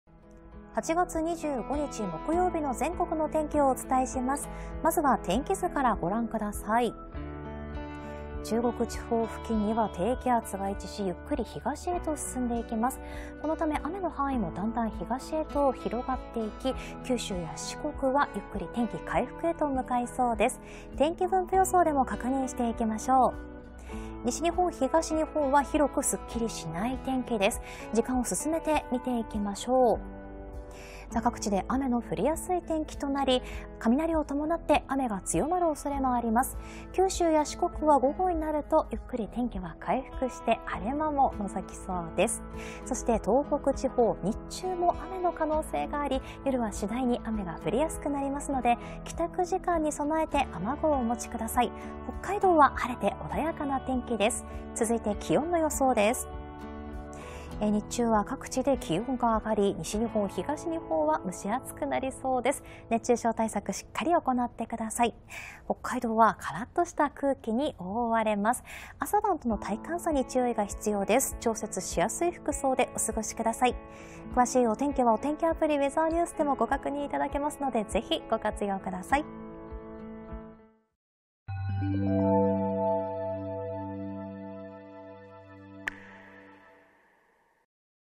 【8月25日(木)の天気】西日本・東日本はスッキリしない空模様
西日本や東日本は湿った空気の影響を受けて、曇りや雨のすっきりしない天気となります。東北は雲が広がりやすいものの、北海道は高気圧に覆われて青空が広がる見込みです。